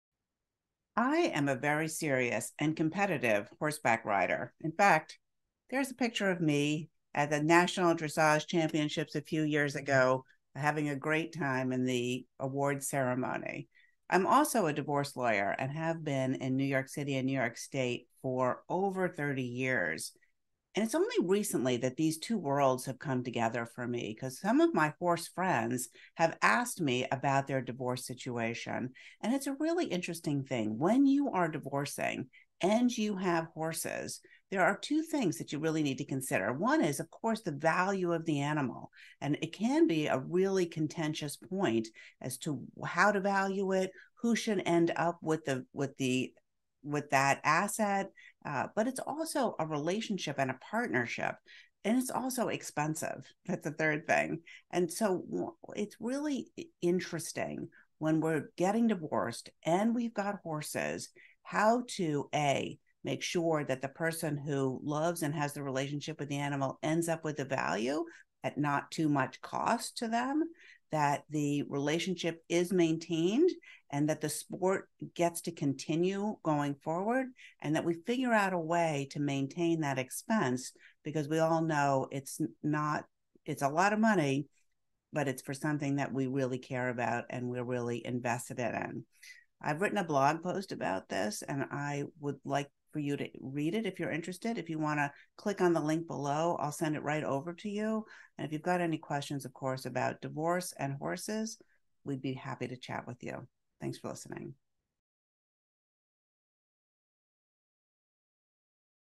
Divorce And Keep The Horse
For more information about keeping your horse after the divorce, follow Katherine's blog

Family Law Attorney Katherine Miller is a horse lover and dressage competitor. Listen to Katherine's advice if you are going through a divorce and want to learn what you can do to make sure you can continue to ride your horses.